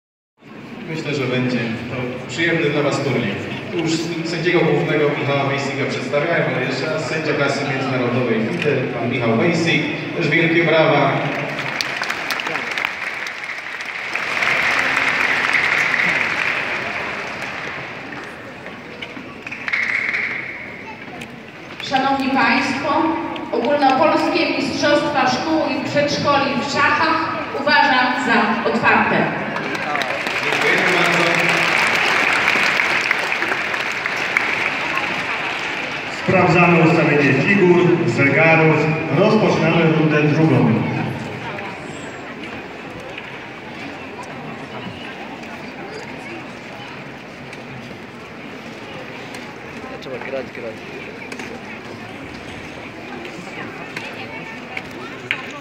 Mistrzostwa Polski Szkół i Przedszkoli Szachy
Szachy Katowice Spodek 2017. Mistrzostwa Polski otwarcie zawodów.